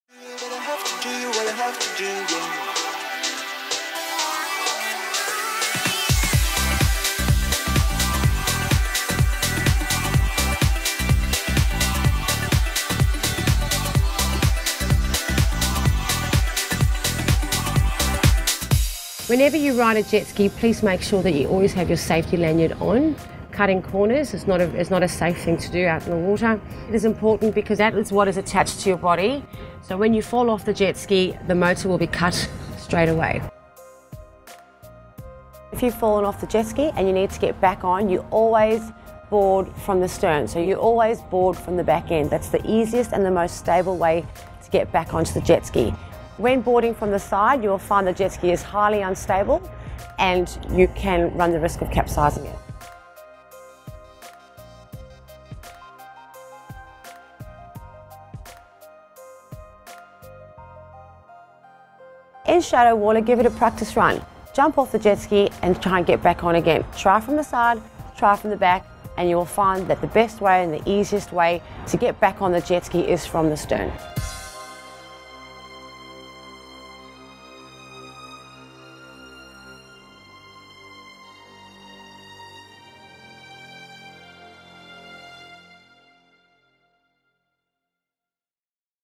Jetski - Episode 05 Getting back onboard after a fall
Ever tried to get back on the ski from the side. I am guessing that it hasn't worked out so well for you then.